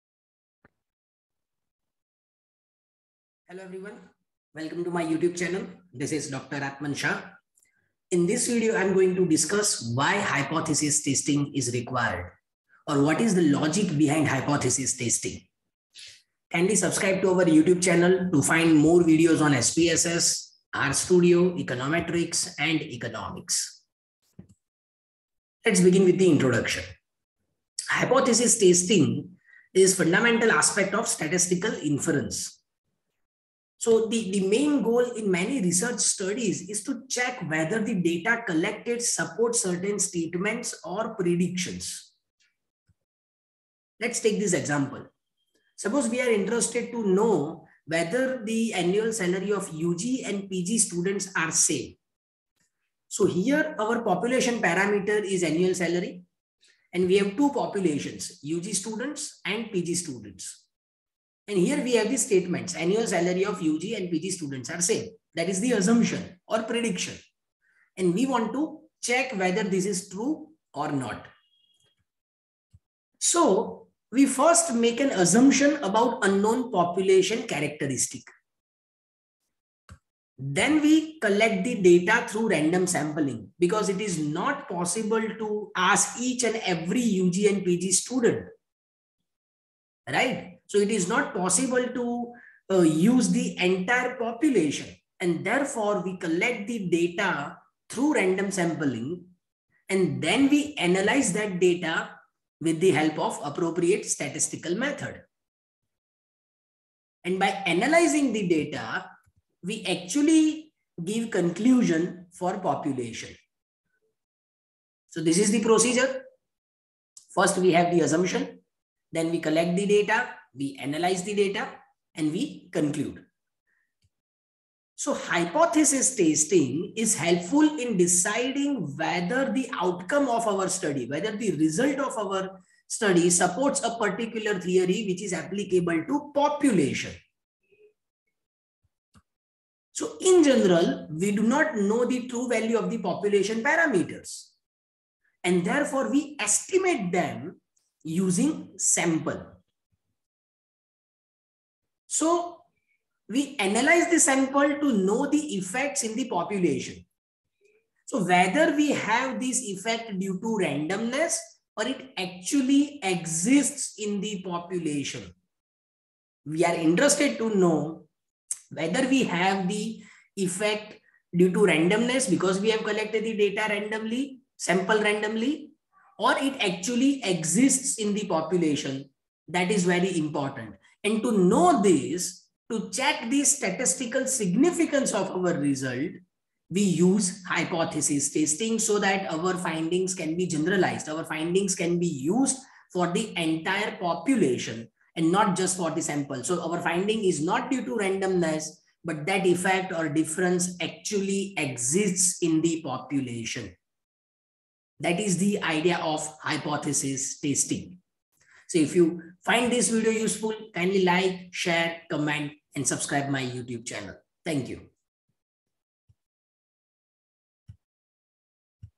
Why Hypothesis testing? || Idea behind hypothesis testing || Statistics || Dr. Atman Shah || SXCA
This video explains the idea behind hypothesis testing using examples.